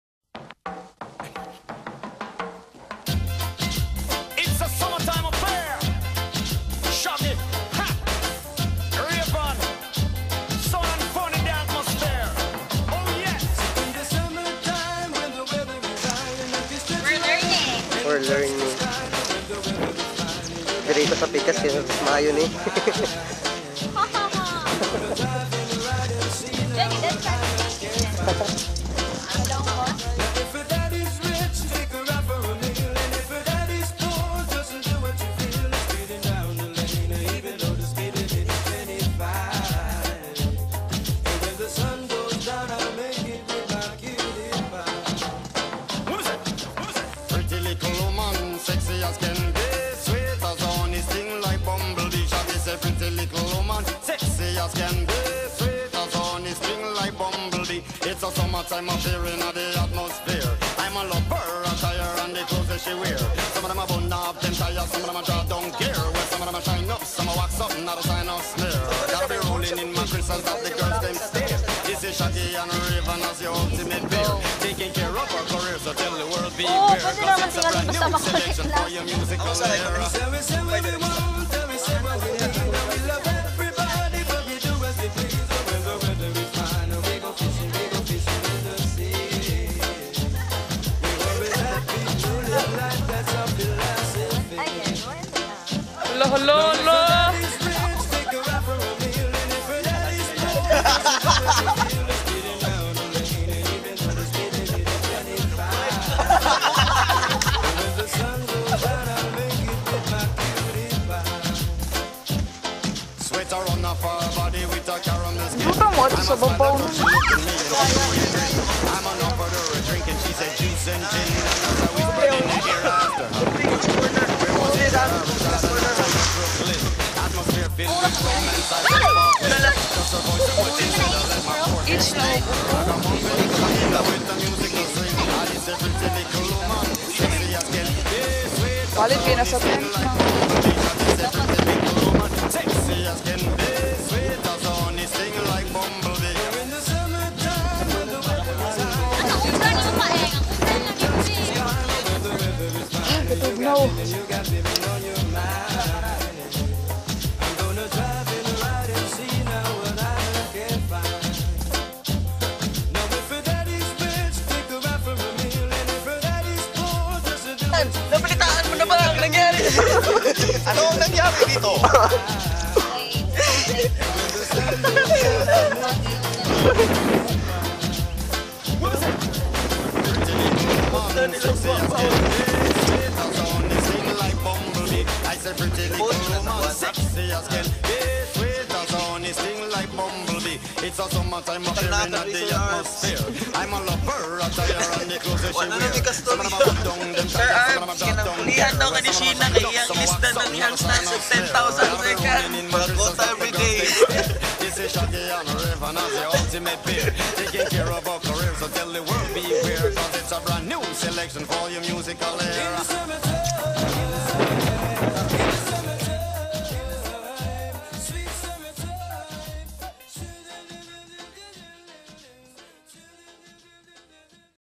Osmeña Peak
Putting up our tents. (May of 2011). Osmeña Peak (Mantalongon, Dalaguete, Cebu)